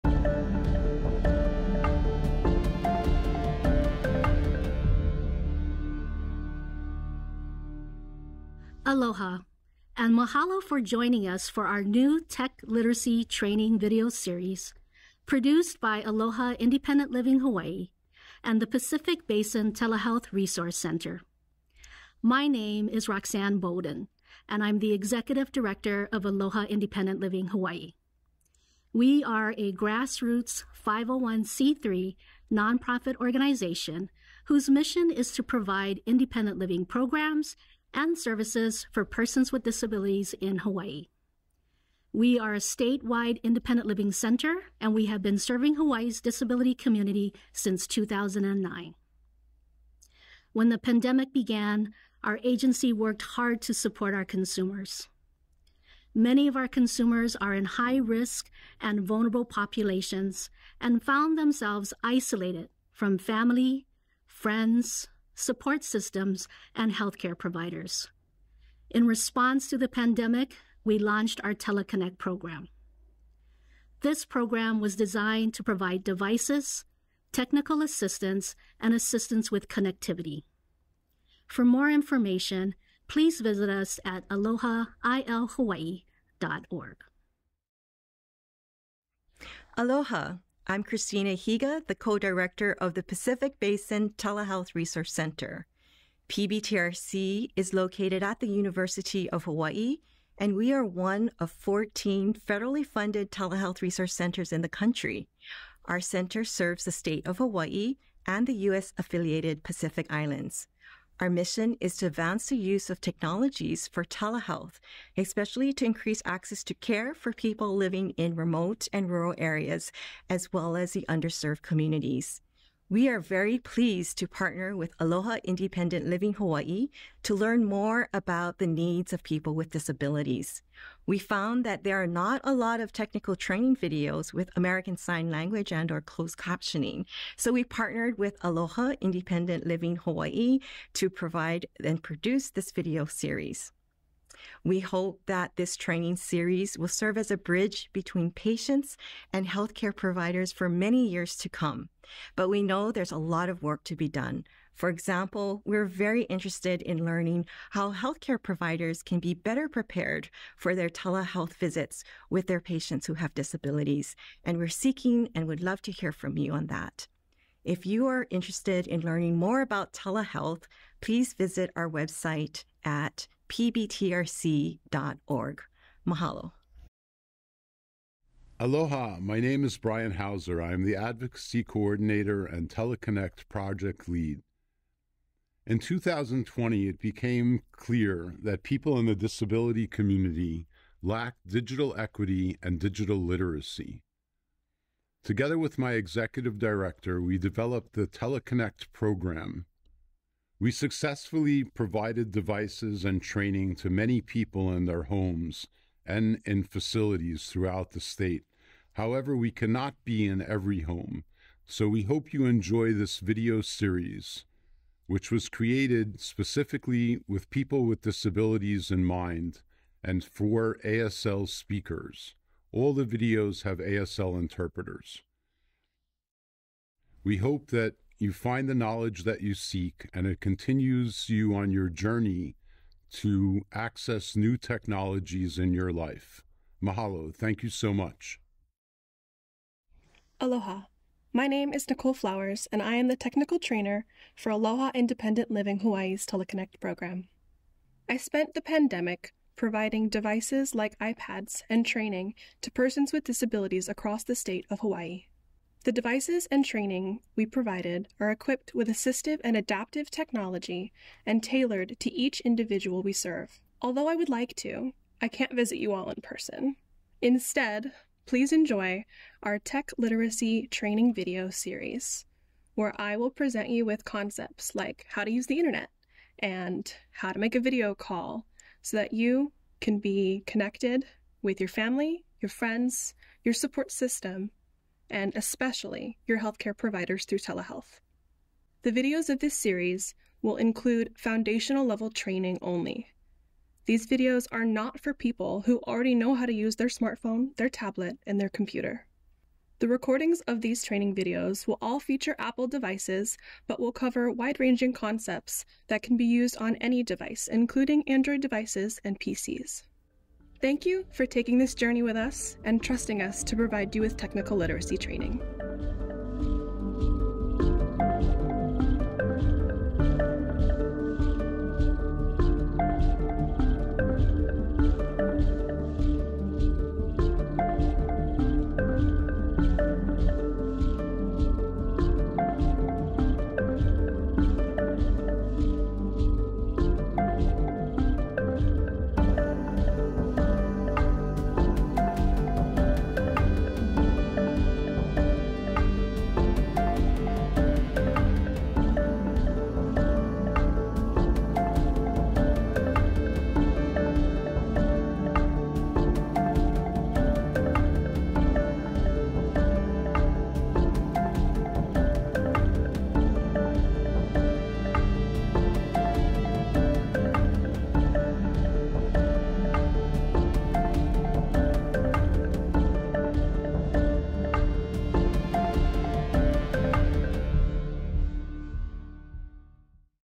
Tech Literacy Training Series Introduction (American Sign Language)
Roxanne Bolden
Executive Director, Aloha Independent Living Hawaii (AILH)

Dr. Christina Higa
Co-Director, Pacific Basin Telehealth Resource Center (PBTRC)

Brian Hauser
Advocacy Coordinator & TeleConnect Project Lead, AILH

Nicole Flowers
Digital Literacy Trainer, AILH